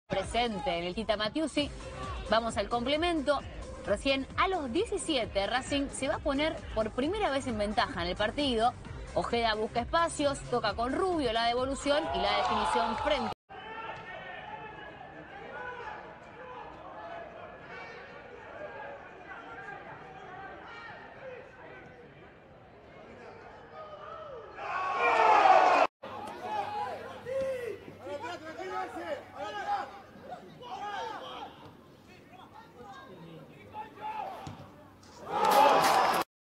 Torneo 2022 - Fecha 8 - Mejores goles
En una fecha repleta de goles y golazos estos fueron, para nosotros, los tres mejores gritos de la fecha 8 del torneo de Reserva

📌 Agustín Ojeda (de Racing a Newell's)
📌 Kevin Isa Luna (de AtleticoTucuman a Aldosivi)
📌 Rodrigo Mosqueda (de RosarioCentral a Independiente)

📹 Crédito: ESPN